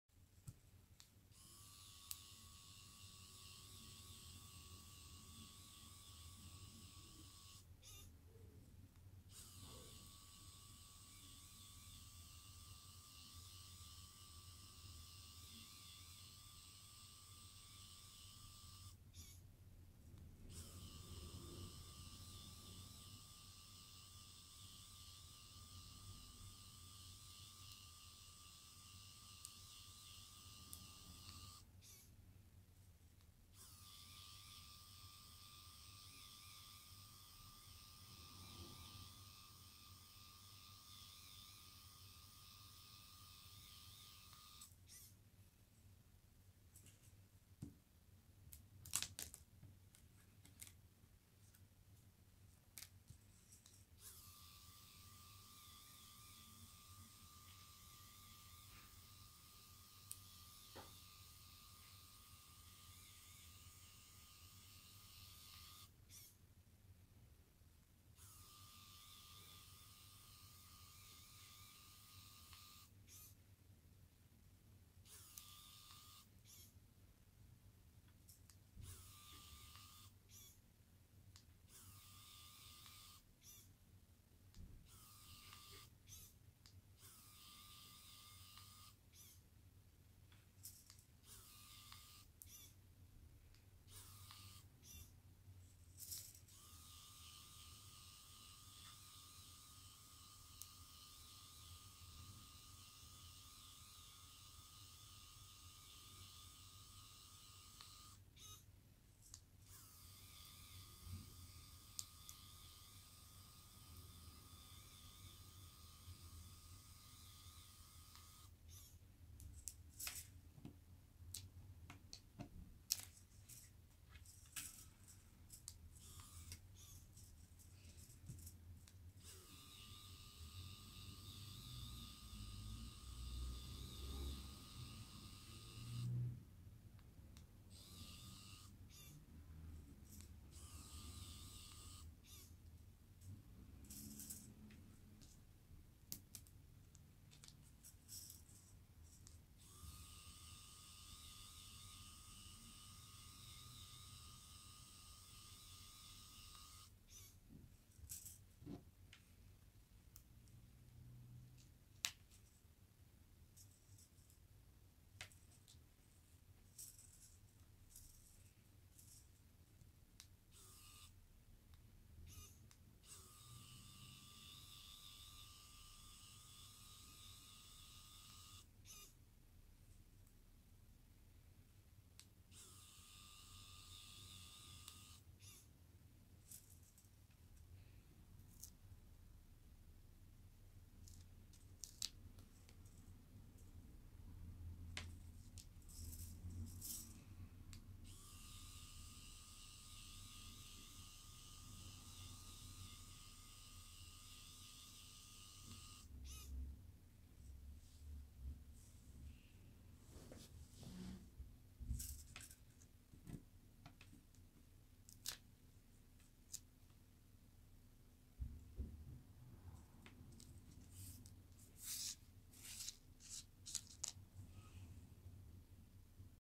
完成まで10分！3Dペンでミニチュア椅子を作る（前編）
3Dペンを使って、ミニチュアの椅子を作ってみました！こちらではパーツを作ります。商品の使い方や、作るときのコツをご紹介している記事はこちら⇒